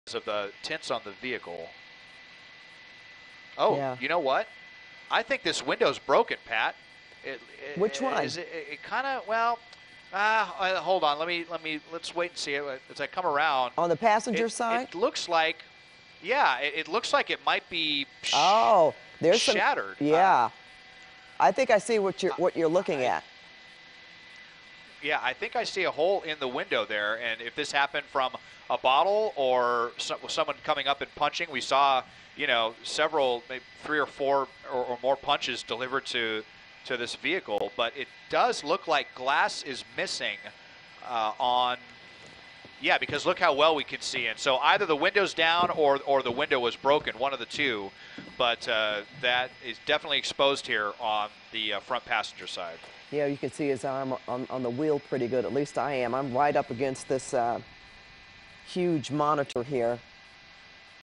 Suspect stops, window appears to be broken
Suspect's passenger-side window appears to be shattered.